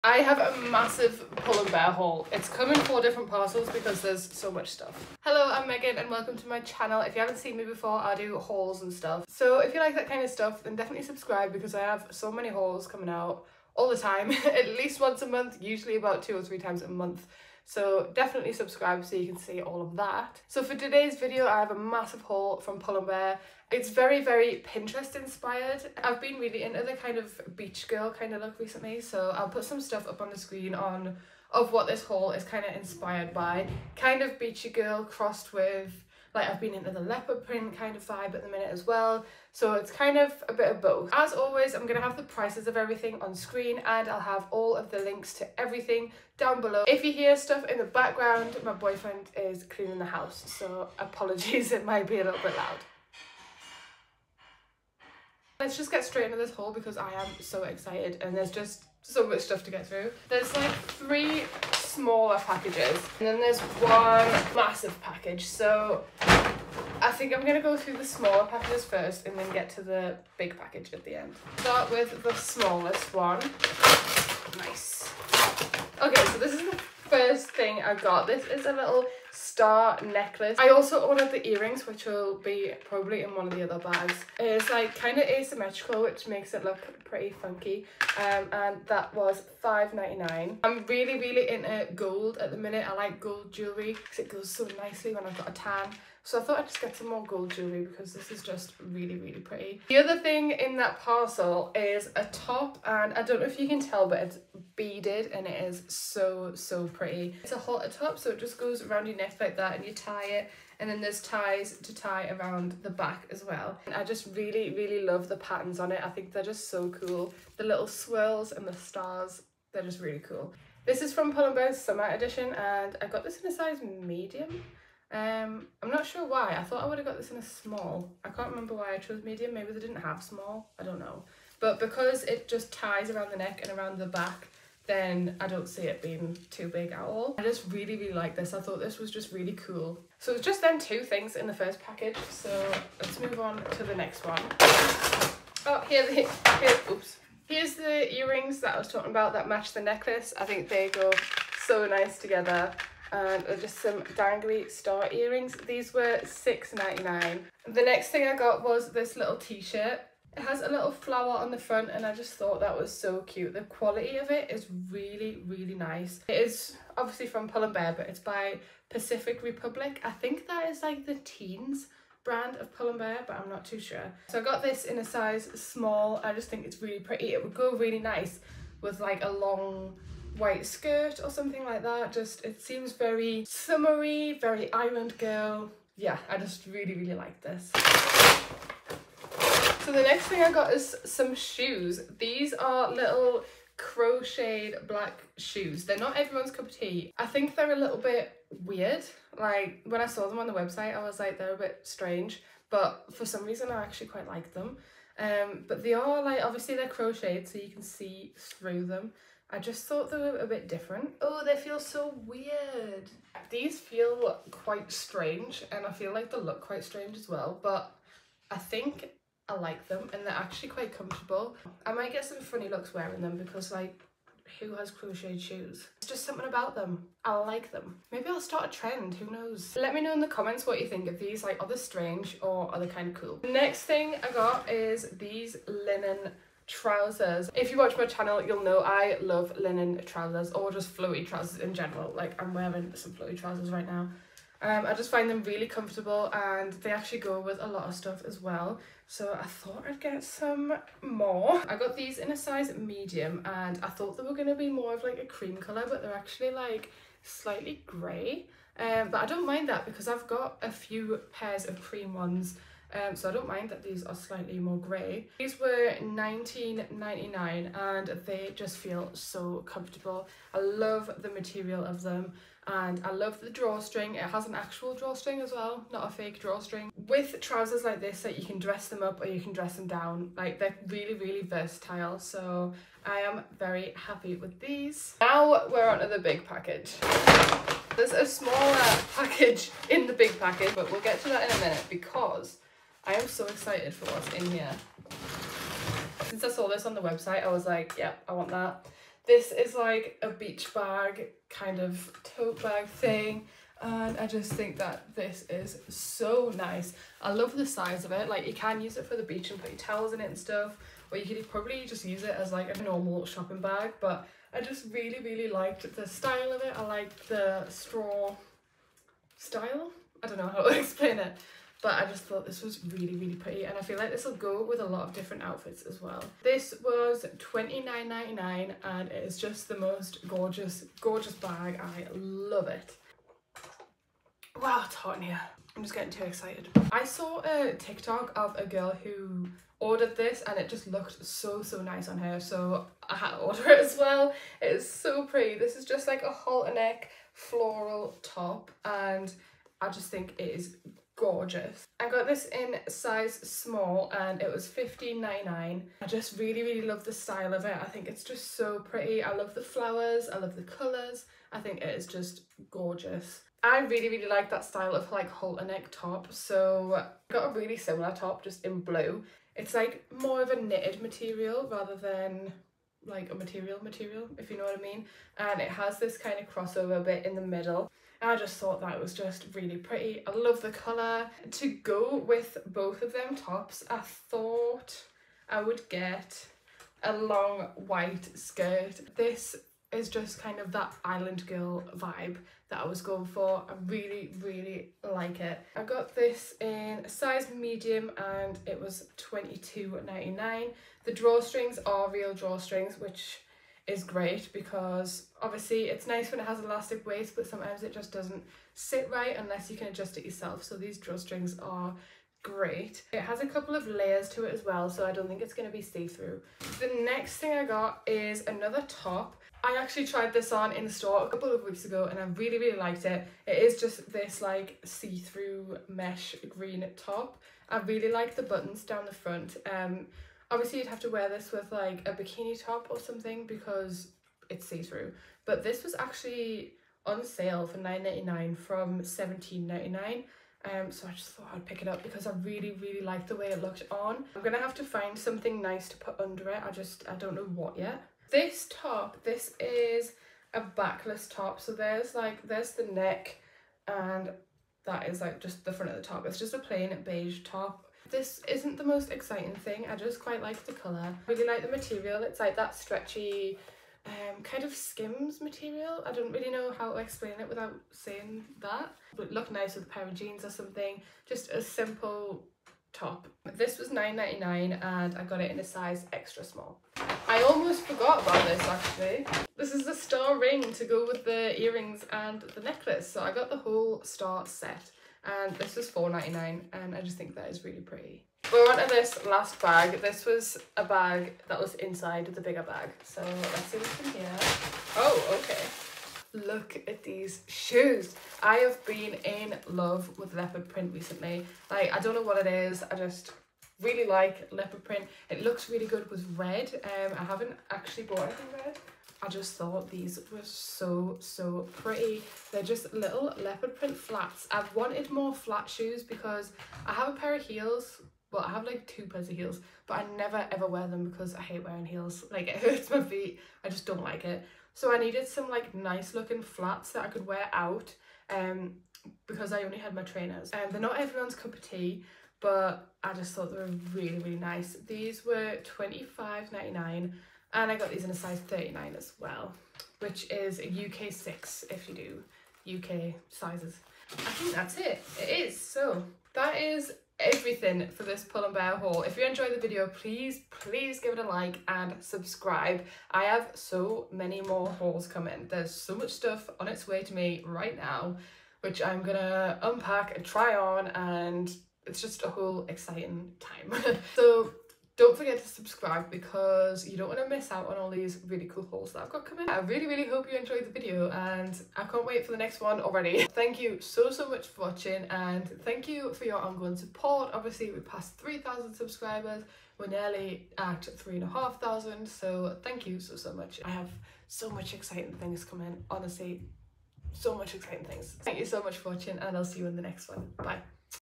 MASSIVE HAUL AUTUMN SEPTEMBER 2024 | Trending Pinterest Inspired | Try-On | Must-Haves
Thank you to Pull&Bear for sending me all of these gorg items!